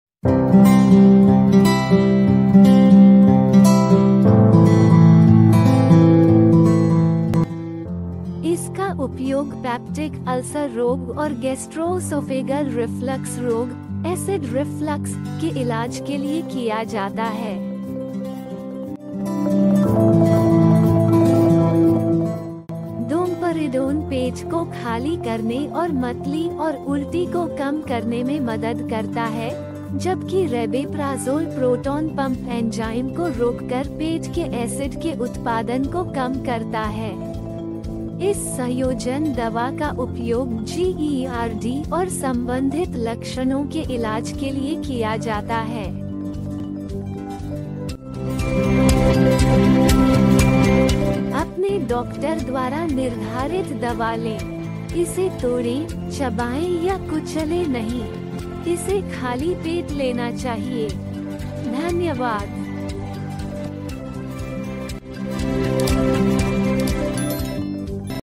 पिवोल टैबलेट | AI Uses, Work and How to take In Hindi.
पिवोल टैबलेट
Composition- डोमपरिडोन (एनए) + रैबेप्राजोल (एनए)

Introduction
इसका उपयोग पेप्टिक अल्सर रोग और गैस्ट्रोओसोफेगल रिफ्लक्स रोग (एसिड रिफ्लक्स) के इलाज के लिए किया जाता है।

डोमपरिडोन पेट को खाली करने और मतली और उल्टी को कम करने में मदद करता है, जबकि रैबेप्राज़ोल प्रोटॉन पंप एंजाइम को रोककर पेट के एसिड के उत्पादन को कम करता है। इस संयोजन दवा का उपयोग जीईआरडी और संबंधित लक्षणों के इलाज के लिए किया जाता है।

अपने डॉक्टर द्वारा निर्धारित दवा लें। इसे तोड़ें, चबाएं या कुचलें नहीं। इसे खाली पेट लेना चाहिए।